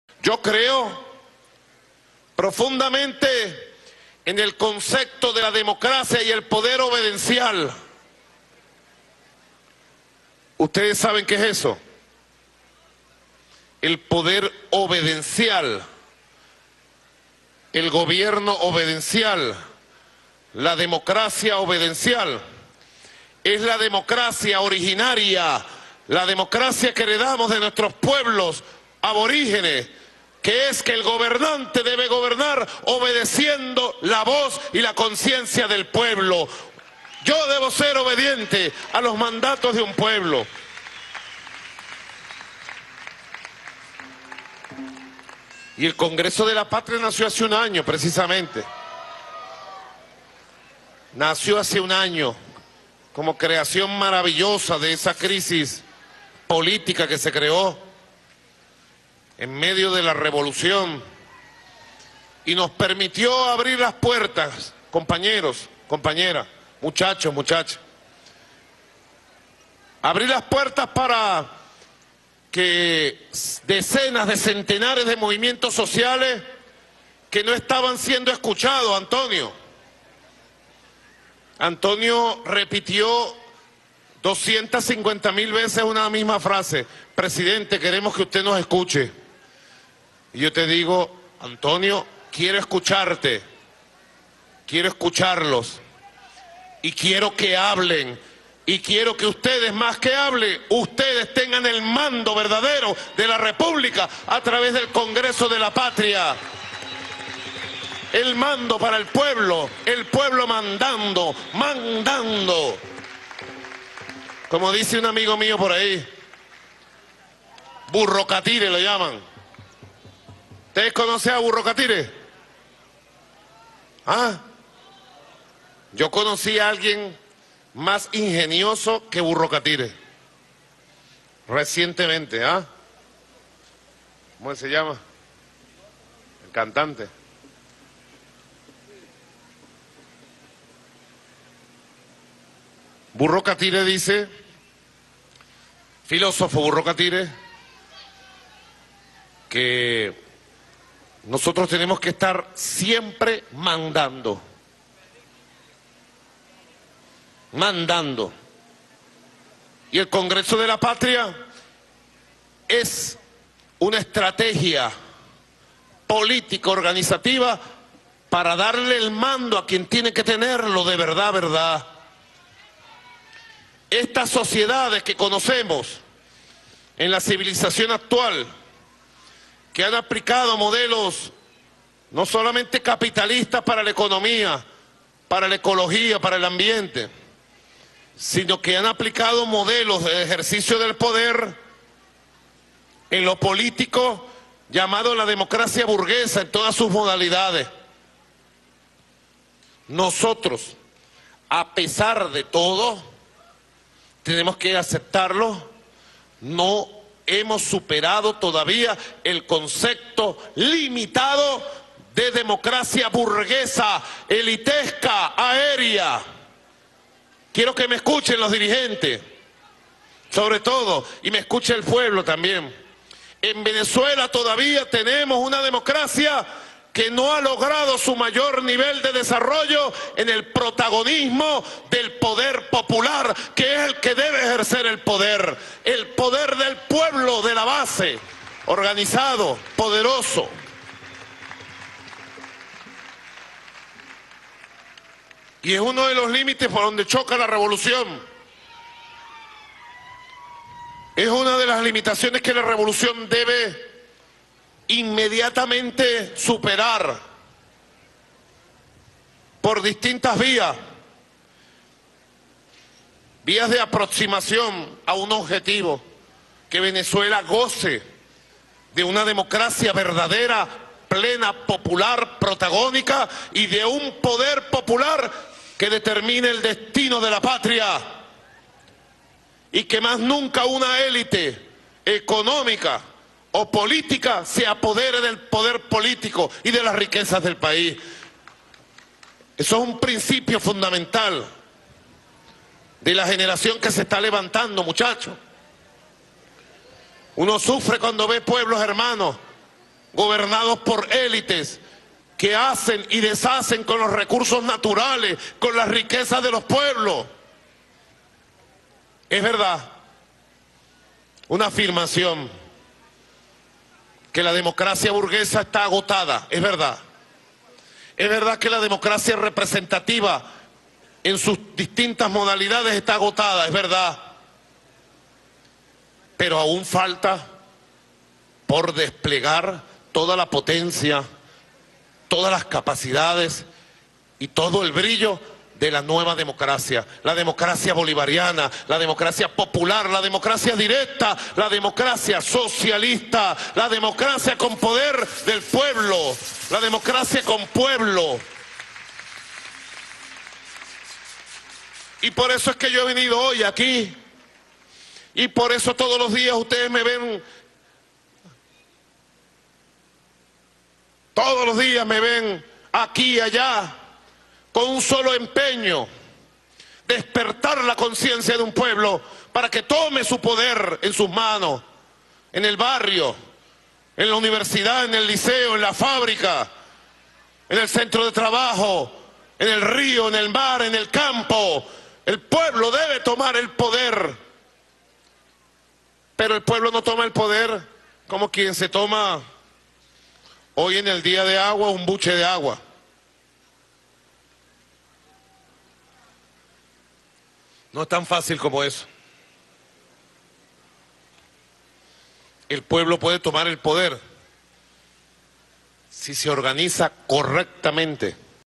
Maduro exige a dirigentes del PSUV superar la democracia burguesa y aérea e ir a la protagónica
"En Venezuela todavía tenemos una democracia que no ha logrado su mayor nivel de desarrollo en el protagonismo del poder popular, que es el que debe ejercer el poder. ¡Poder del pueblo, de la base, organizado, poderoso! Es uno de los límites donde choca la revolución. Es una de las limitaciones que la Revolución debe superar inmediatamente", señaló el Presidente Nicolás Maduro este 22 de marzo de 2017 en una reunión del Congreso de la Patria, Capítulo Ecosocialismo. "Que más nunca una cúpula económica o política se apodere del poder político y de las riquezas del país".

Espera que la democracia bolivariana, popular, directa y socialista finalmente termine imponiéndose.